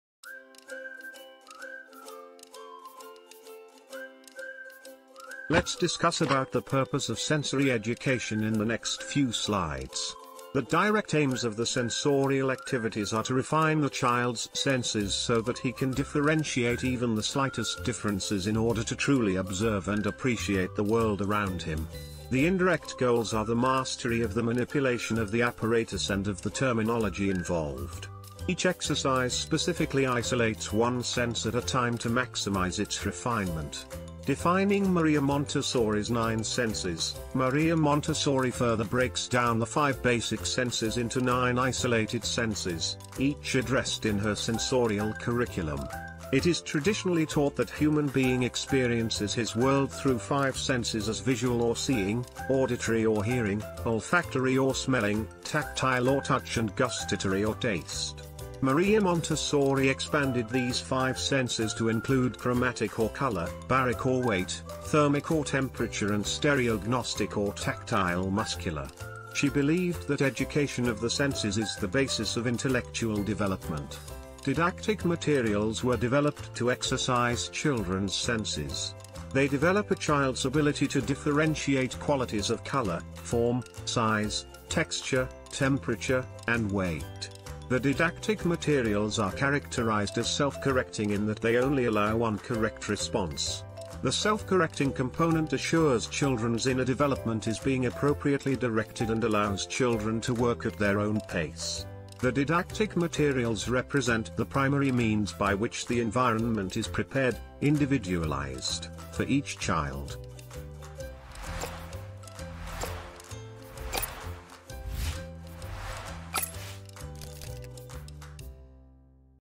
Purpose of Sensory Education
The sensory educational approach by Maria Montessori focuses on the purpose of sensory education to provide children good growth opportunities.